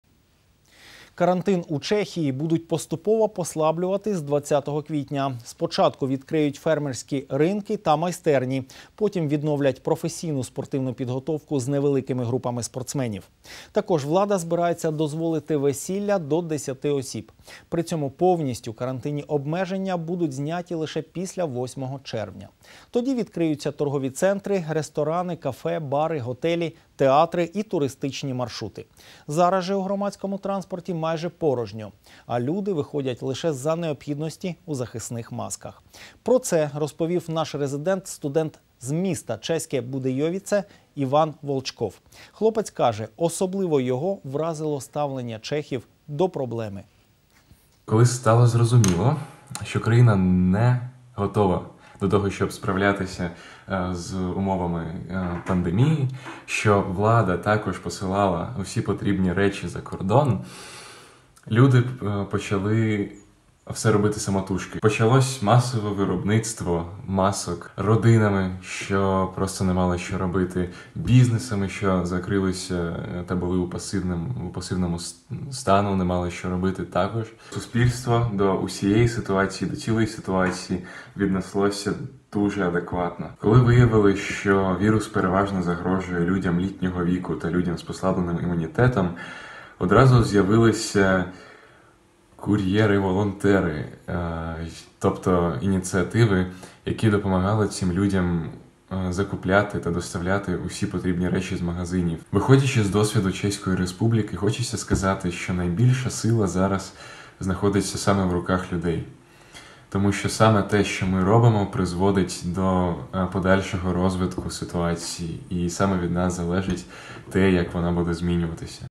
Коронавірус в Європі: Чехія – ще одна країна, яка послаблює карантин
Карантин у Чехії будуть поступово послаблювати з 20 квітня. Спочатку відкриють фермерські ринки та майстерні, потім відновлять  професійну спортивну підготовку з невеликими групами спортсменів. Також влада збирається дозволити весілля — до 10 осіб. При цьому повністю карантинні обмеження будуть зняті тільки близько 8 червня. Тоді відкриються торгові центи, ресторани, кафе, бари, готелі, театри і туристичні маршрути. Зараз же у громадському транспорті майже порожньо, а люди виходять лише за необхідності — у захисних масках. Про це розповідає наш резидент, студент з міста  Чеське Будейовіце Іван Волчков. Хлопець каже: особливо його вразило ставлення чехів до проблеми.